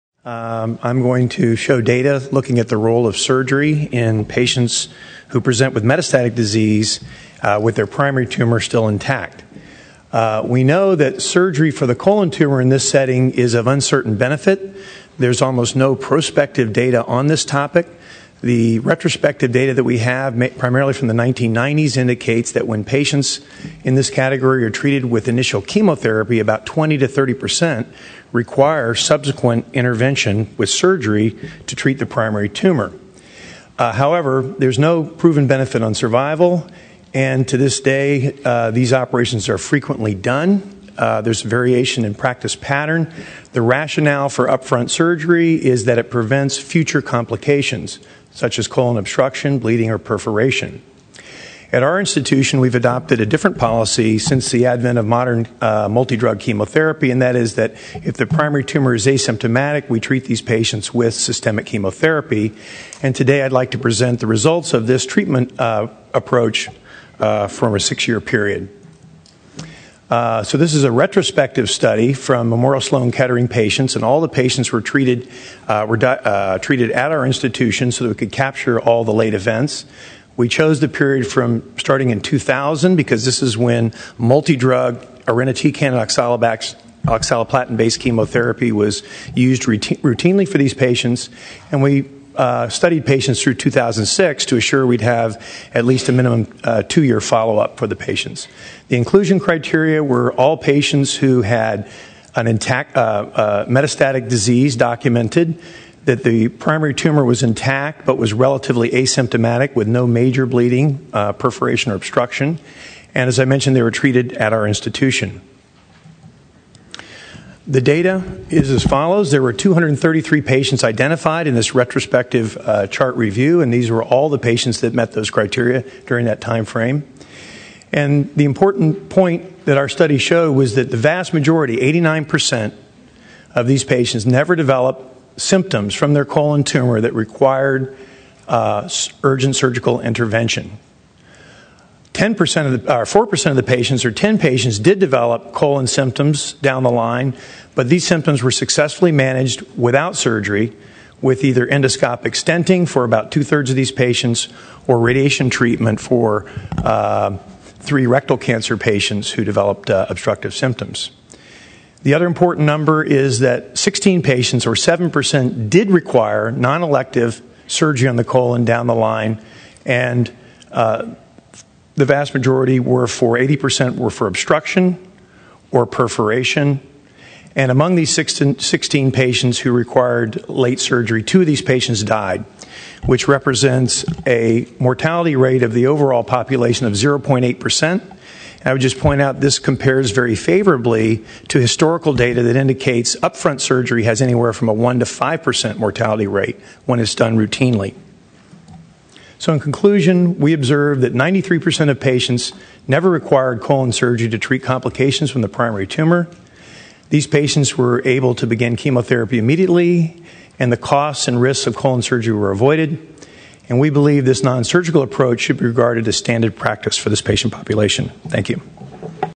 ASCO: Surgery unnecessary for majority of patients with advanced colorectal cancer
Dr. Philip B. Paty - Memorial Sloan-Kettering Cancer Center, New York, USA - speaking at ASCO 2009 press conference: New research shows that patients who are newly diagnosed with metastatic, surgically incurable colorectal cancer do not need immediate surgery to remove their primary tumour unless the tumour is causing complications.